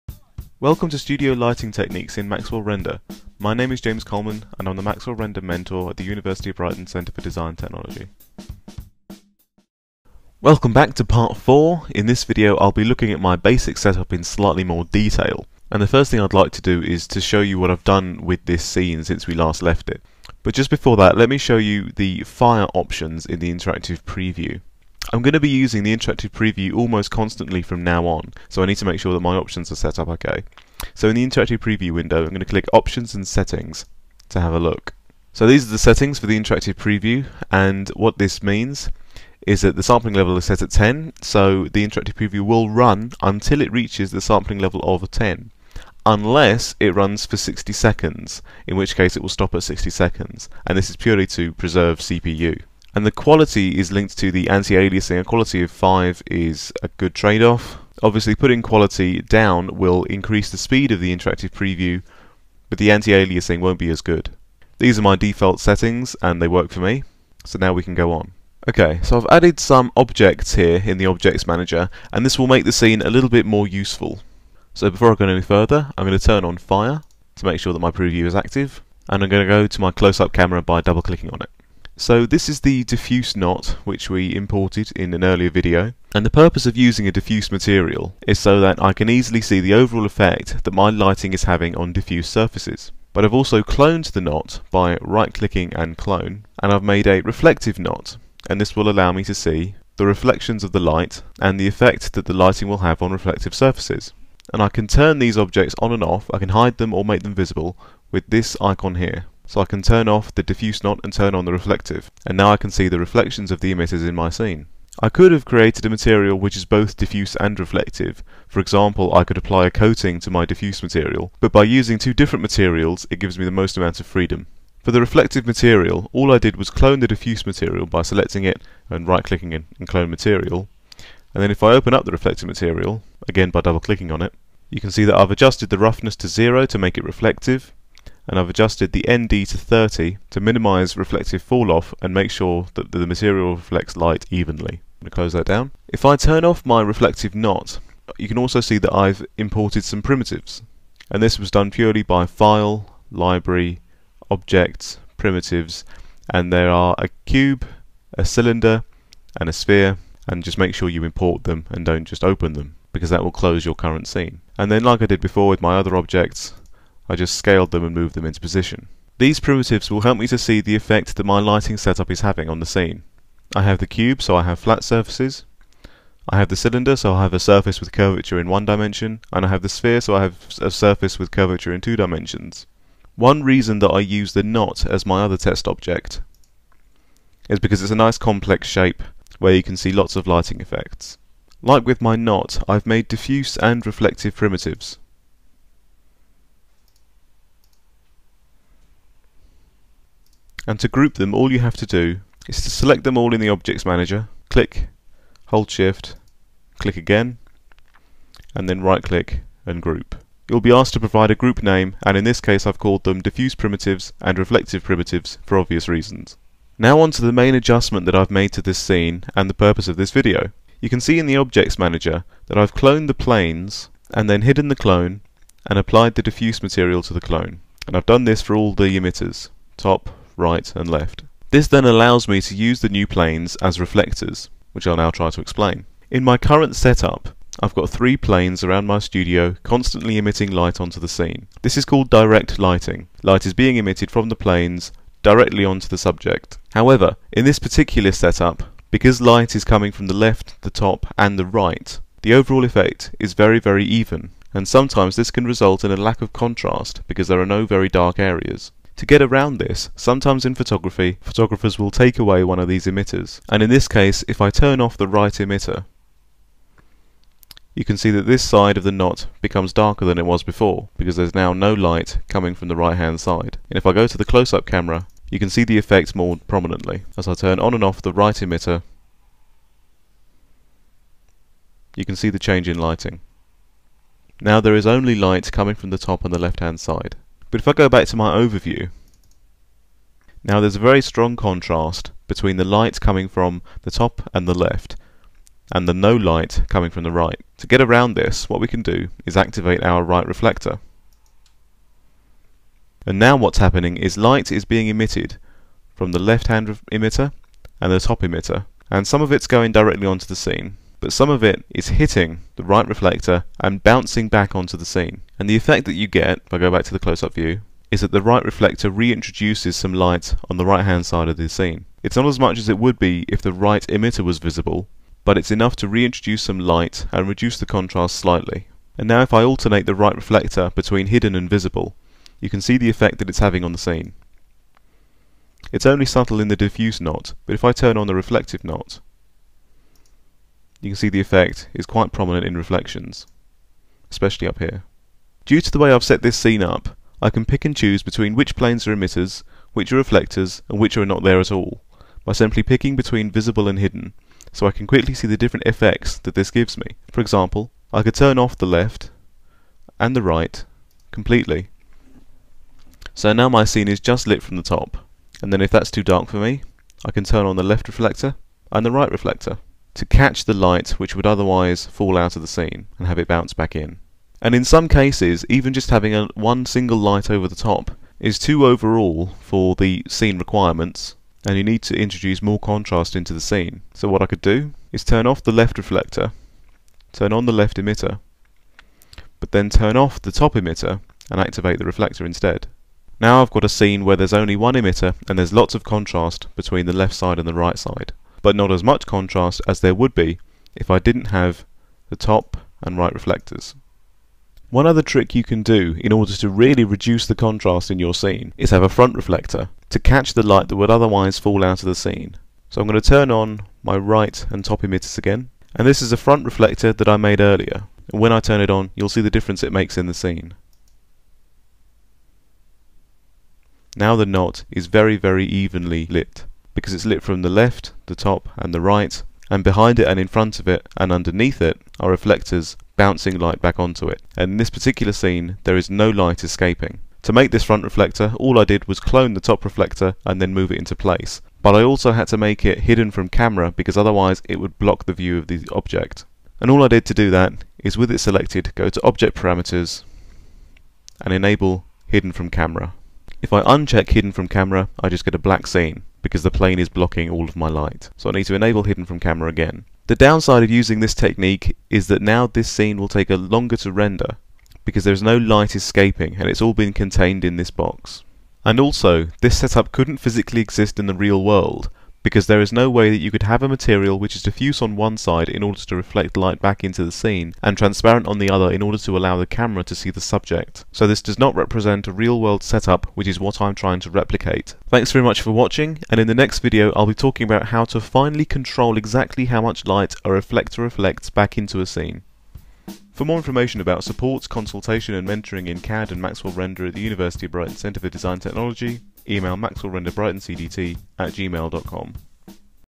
Studio Lighting Techniques Part 4: Setup Detail
This video is a more in depth analysis of my scene, and I start with a review of my FIRE (interactive preview) settings, before showing what I've done to my scene to make it more useful (simply cloning planes to make reflectors). Then I've explained the point of making reflectors by talking about direct and indirect lighting, and shown how turning emitters and reflectors on and off can lead to different results. I've also introduced some new test objects: a reflective knot, and some diffuse and reflective primitives.

The Centre for Design Technology staff and students provide courses, equipment, consultancy and research in Product design at the University of Brighton.